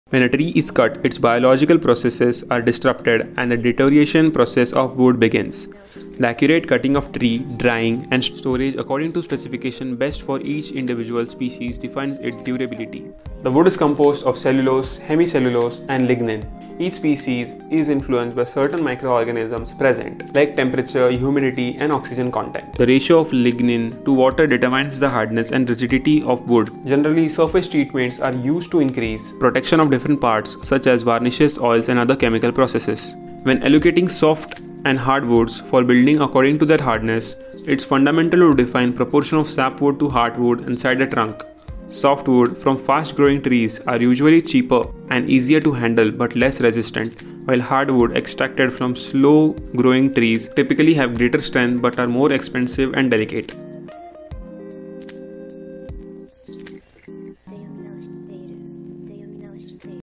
What's Wood?
This video is about what processes take place from the felling of the tree to selling it for construction. The ratio of water to ligin (a major categorized element in wood) defines the use of wood.

The wood is composed of cellulose, hemicellulose, and ligin each species is influenced by certain microorganisms present, temperature humidity, and oxygen content. The ratio of ligin to water determines the hardness and rigidity of the wood.

The difference between polished and unpolished wooden surfaces is mentioned here as well finishing with softwood and hardwood and its various occurances